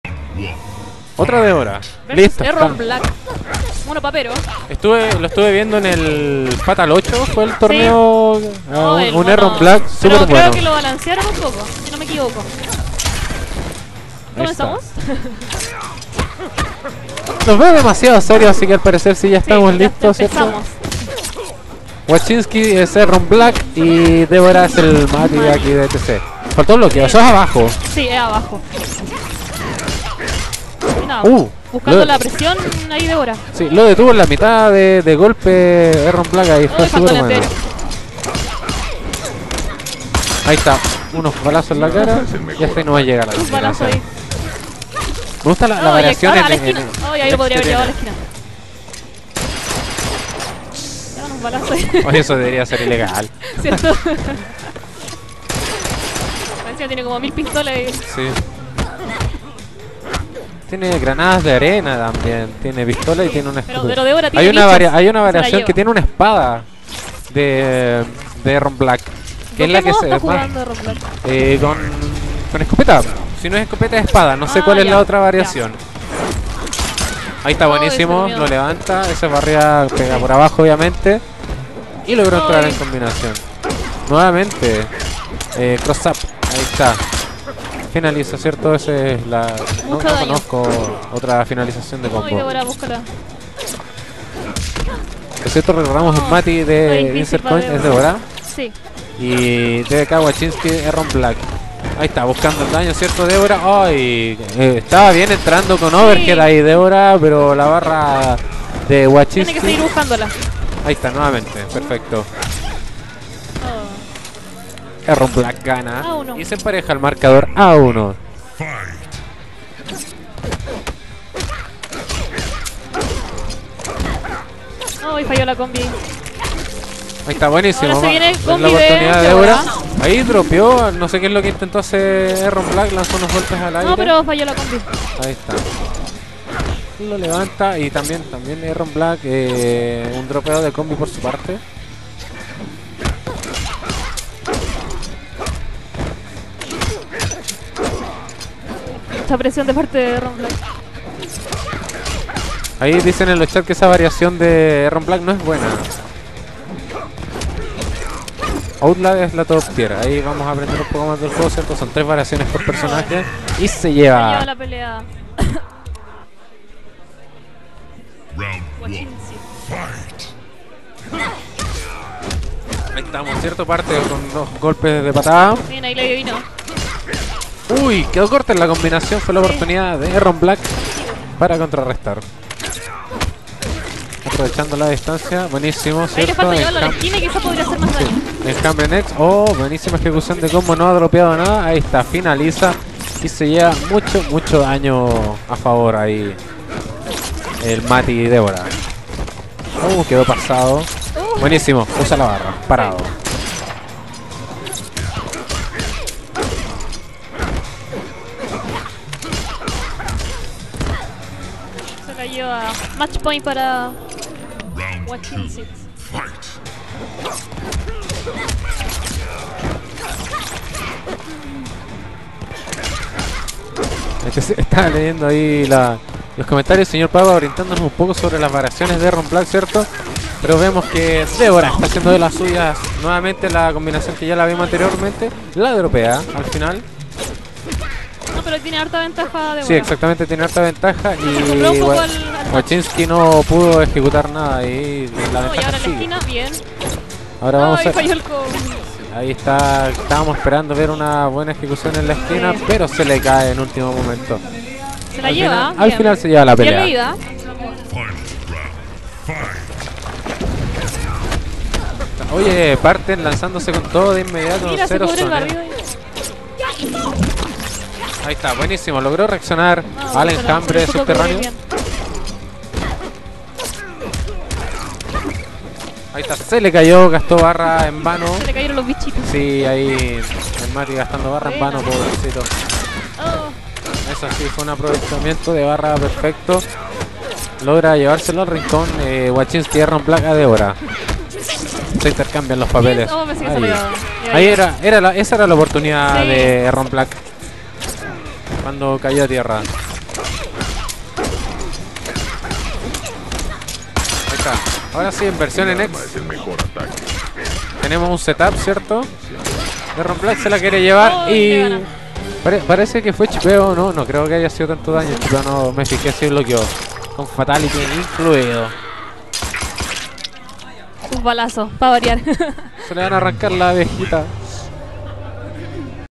BDK Washincitz (Erron Black) Vs IC Mati (D'Vorah) 12° Drinks & Fight Tournament Series
UNIRSE A LA MAYOR COMUNIDAD DE TORNEOS DE FIGHTING GAMES

12°

Lugar:

Insert Coin Bar:
Lugar: Av. Irarrázaval 1920, Ñuñoa, Santiago.

Premios: Insert Coin Bar.